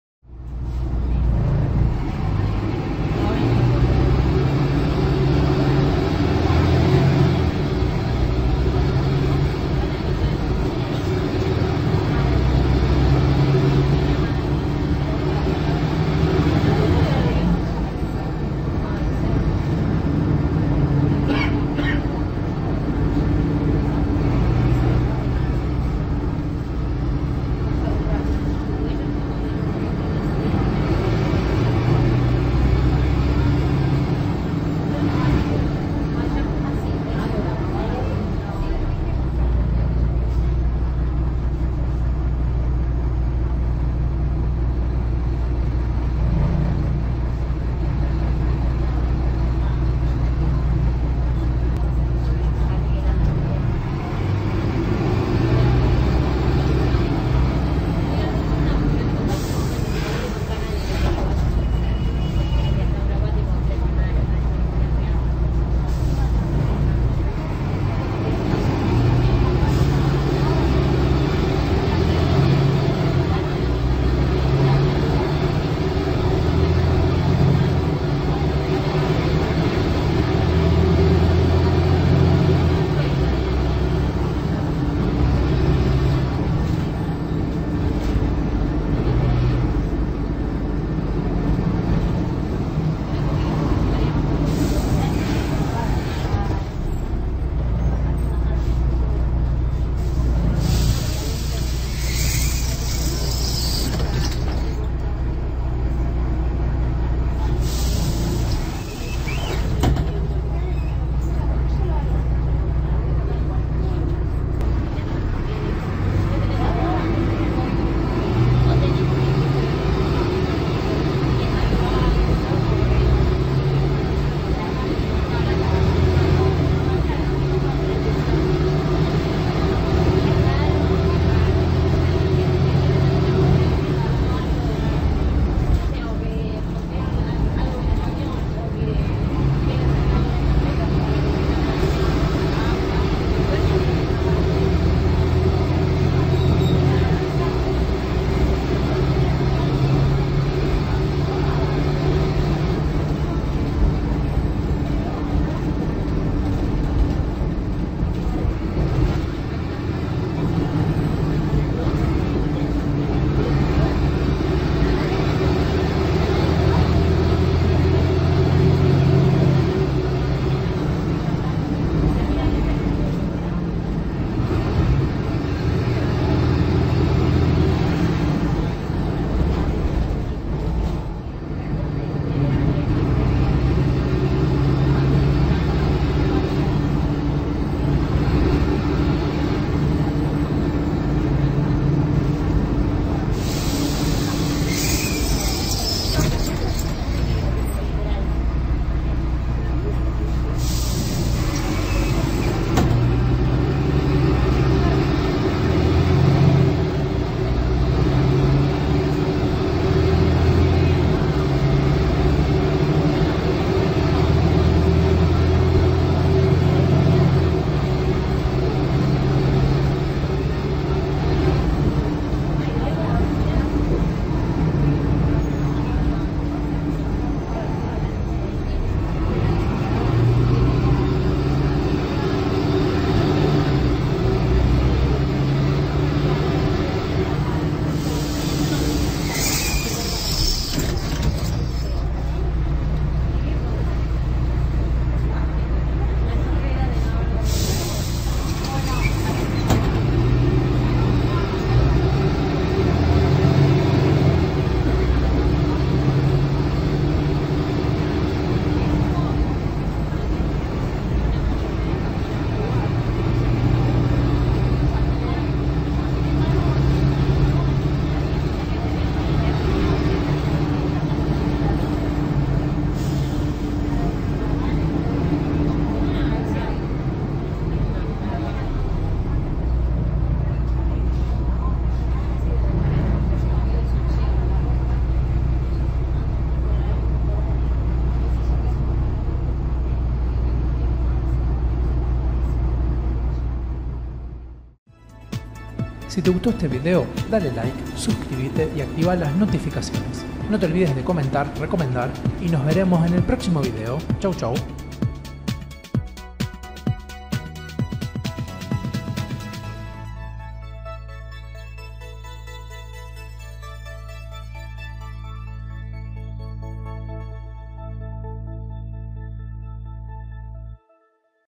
🎧 ERSA - 3733 (Volksbus OT18.280)
📺 Impactante Grabación de un Volksbus Ronco con Escape Libre que te invitamos a Disfrutar con la Magia y el Estilo de Sonibus.

⚙ |DATOS TÉCNICOS| ⚙
🚍 LINEA: 133
🔢 INTERNO: 3733
🏁  EMPRESA:  ERSA
🎫 SERVICIO: Urbano
💳 MEDIOS DE PAGO: Tarjeta SUBE
⚙ CHASIS: Volksbus OT18.280
🔩 EQUIPACIÓN: Man D8
☘ NORMA DE EMISIÓN: Euro 5
🎚 TRANSMISIÓN: Automático
🎚 CAJA AUTOMÁTICA: ZF EcoLife
🎚 CANTIDAD DE MARCHAS: 6
🛢 CILINDROS EN LÍNEA: 6
⛓ POSICIÓN DEL MOTOR: Atrás
🔨 CARROCERÍA: Metalpar
🌀 MODELO: Nueva Generación
💺 CAPACIDAD DE ASIENTOS: 27
🛌🏼 TIPO DE ASIENTOS: Urbanos
🛋 MARCA COMERCIAL: Gia Flex
☀❄ UNIDAD CLIMATIZADA: Si
♿ ACCESO A LA UNIDAD/PISO: Super Bajo
📅 AÑO APROXIMADO DE FABRICACIÓN: 2018
🎬 EDICIÓN: Grupo Sonibus
💾 FECHA DE GRABACIÓN: Octubre 2024
🎤 MEDIO DE GRABACIÓN: Celular
📱 MARCA: Samsung Galaxy A32
🎬 MATERIAL: Grupo Sonibus